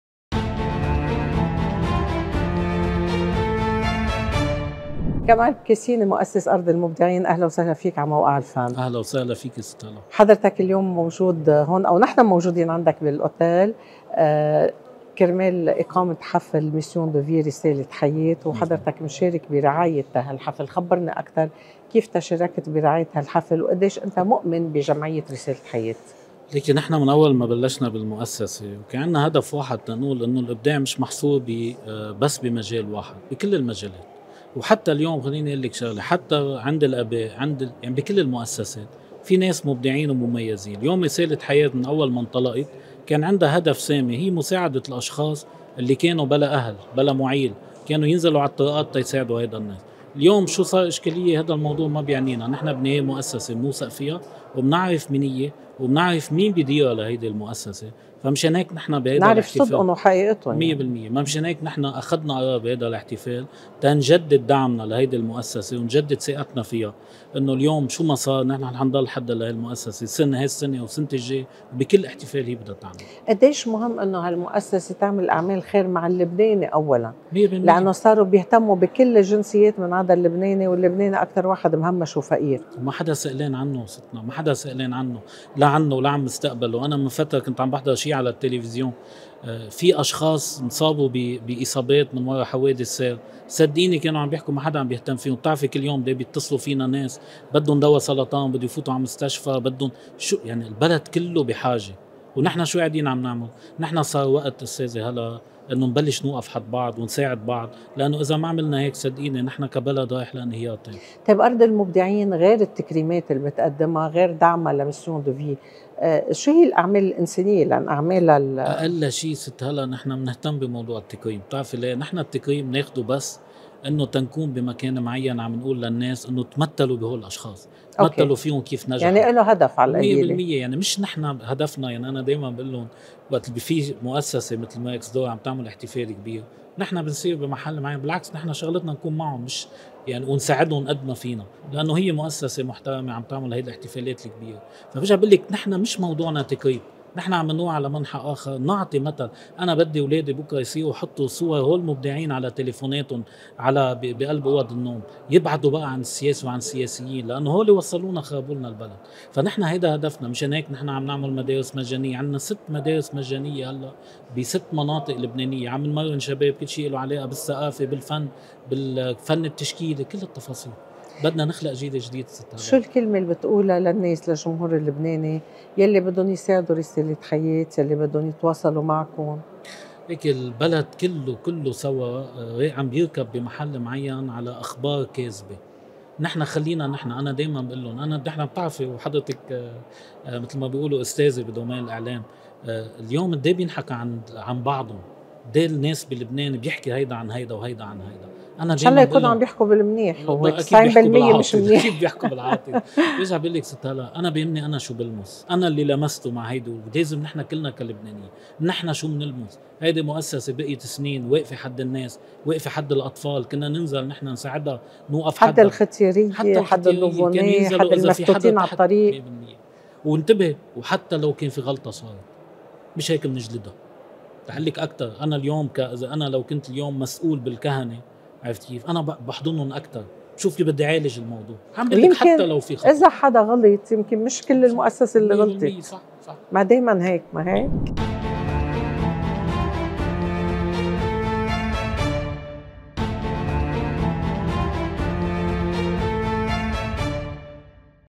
خاص-كمال بكاسيني يشرح سبب تركيز أرض المبدعين على التكريم وهذا ما قاله عن التعاون مع جمعية رسالة حياة
تعاون مؤسس "أرض المبدعين" كمال بكاسيني مع جمعية رسالة حياة العالمية، التي أسسها الأب وسام معلوف، لإقامة احتفال بمناسبة اليوبيل الفضي 25 سنة على تأسيس رسالة حياة، وقد أقيم الإحتفال بتاريخ 13 نيسان/أبريل، بحضور العديد من الشخصيات والنجوم والإعلاميين، وقدمت الحفل الزميلة نوال ليشع عبود.
وفي الإحتفال تم توزيع أوسمة تكريمية تقديراً ومحبة لكل من رافق هذه الجمعية ودعمها.
وأدلى النجم وسام حنا بشهادة حياة في بداية الحفل، بالإضافة الى أشخاص من مختلف فئات المجتمع.
وأجرت رئيسة تحرير موقع الفن الاعلامية هلا المر، العديد من اللقاءات، من بينها لقاء مع مؤسس "أرض المبدعين" كمال بكاسيني، الذي تحدث عن مدى إيمانه بجمعية رسالة حياة، وعن الأعمال الإنسانية التي تقدمها مؤسسة أرض المبدعين، وغيرهما من الأمور.

Copyright : ElFann.com